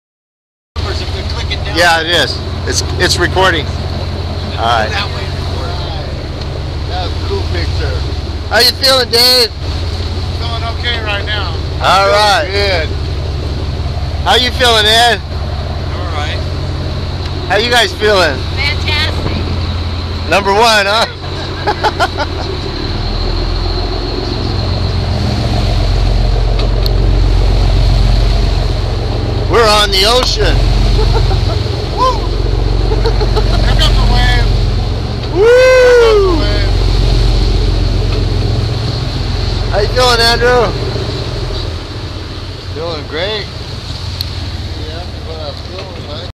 Fort Bragg,Ca Fishing 2014
Fishing on the Trek 2 Charter boat